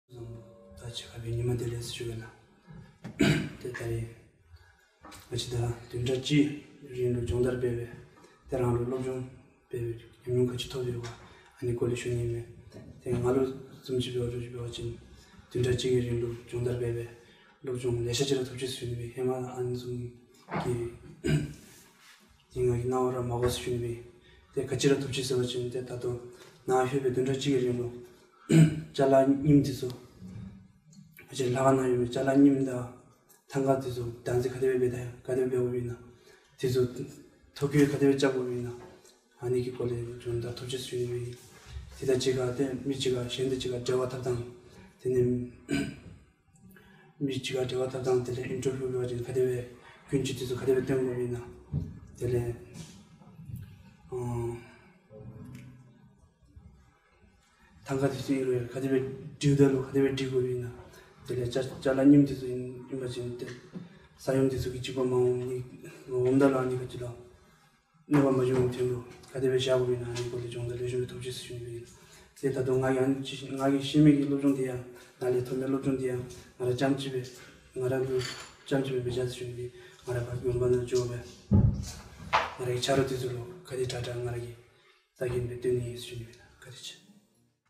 8 Buddhist Monk Talks about Preservation of Monastery Treasures Workshop
Senior Buddhist monk describes what he gained during workshop on Preservation of Monastery Treasures
"After attending the Treasure Caretaker Training for a week I have gained so much knowledge and experience...over the week of training, I learned how to care and keep records of the treasures and artifacts of our monastery and how to conduct interviews." "When I return to my monastery...I want to share the knowledge with my fellow monks."